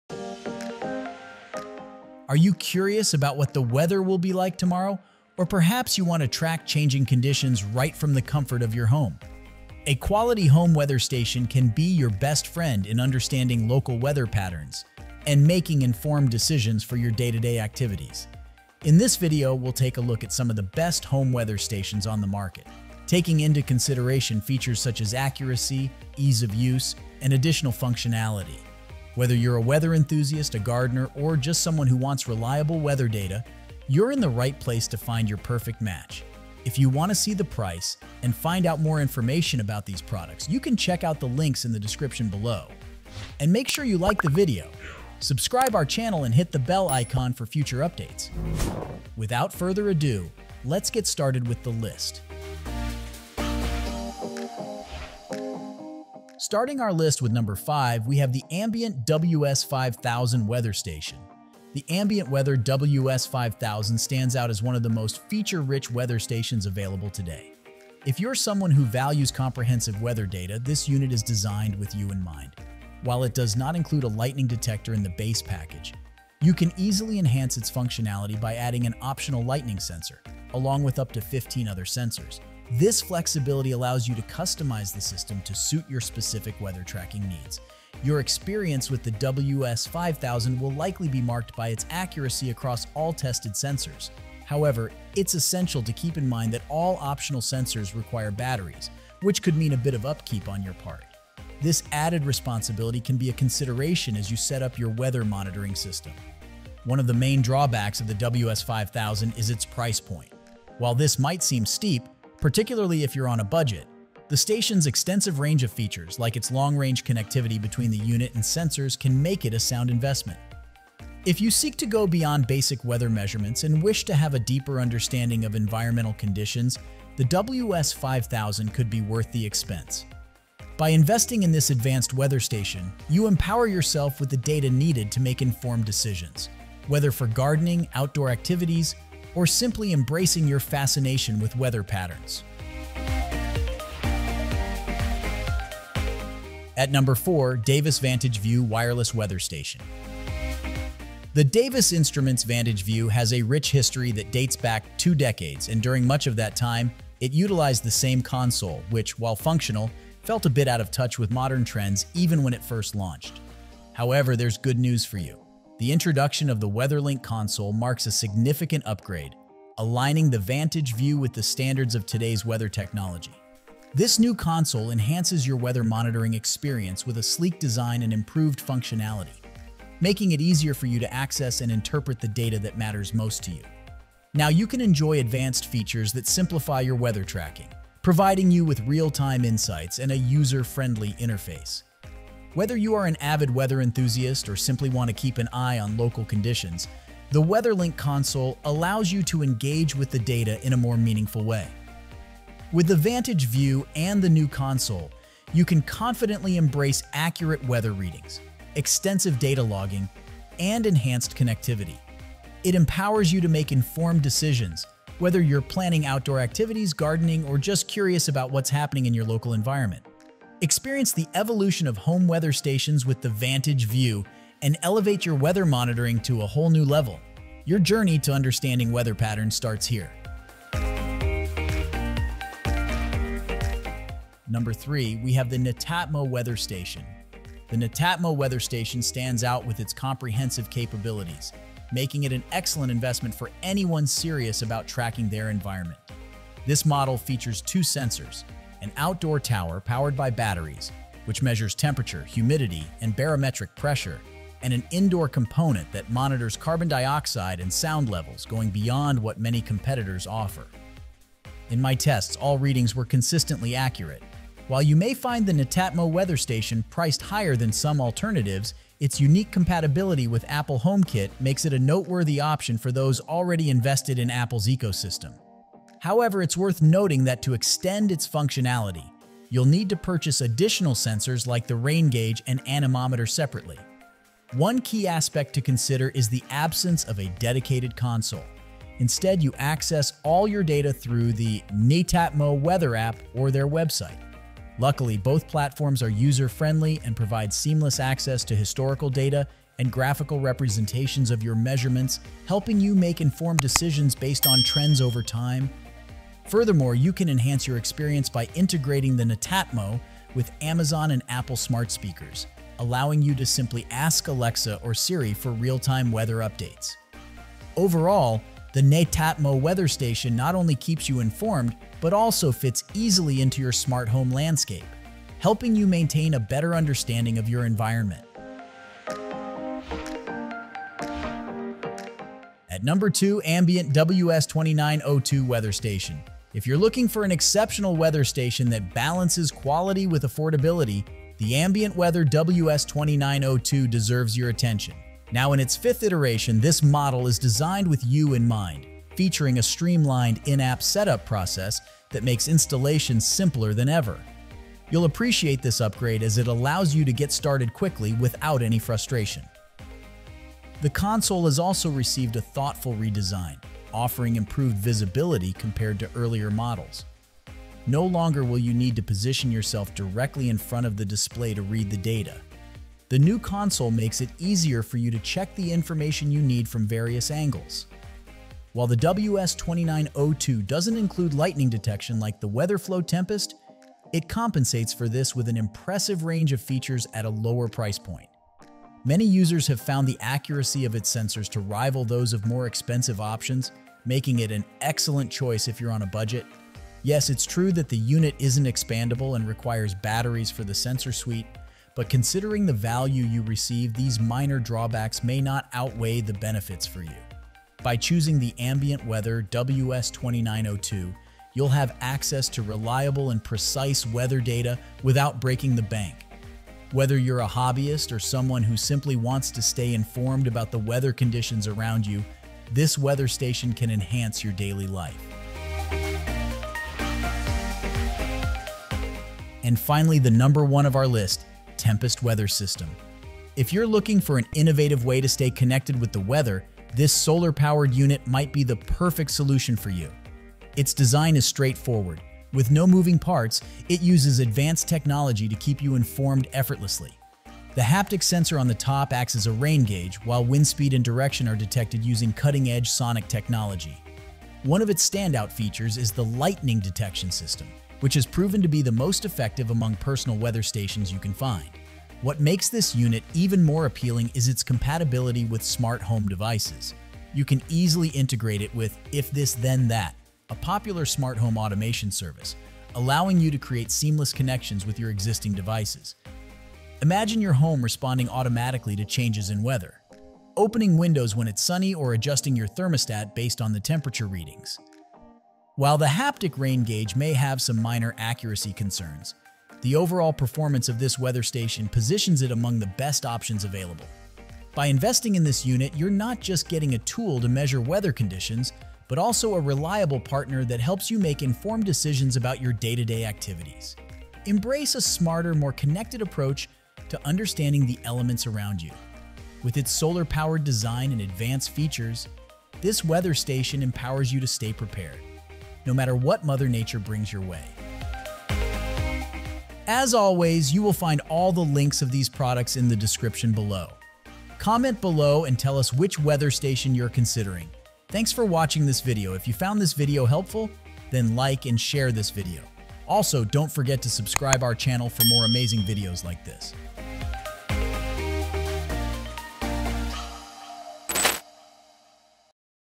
Best Home Weather Stations [2025] - Top 5 Picks
✅ List of the Best Home Weather Stations we featured in this video

1. Tempest Weather System

2. Ambient WS-2902 Weather Station

3. Netatmo Weather Station

4. Davis Vantage Vue Wireless Weather Station

5. Ambient WS-5000 Weather Station

Discover the top home weather stations that offer precision and reliability for your personal weather monitoring needs. The Tempest Weather System leads the way with its advanced technology, providing hyper-local forecasts and real-time data. For a feature-rich experience, the Ambient WS-2902 Weather Station is an excellent choice, offering comprehensive monitoring and integration with smart home systems. The sleek and stylish Netatmo Weather Station stands out with its user-friendly design and ability to monitor both indoor and outdoor environments. The Davis Vantage Vue Wireless Weather Station delivers robust performance with its professional-grade sensors and durable construction, making it ideal for serious weather enthusiasts. Finally, the Ambient WS-5000 Weather Station combines high-end features with wireless capabilities, ensuring you stay informed about the weather conditions around your home. Each of these weather stations offers unique benefits, making them perfect for anyone looking to stay ahead of the weather.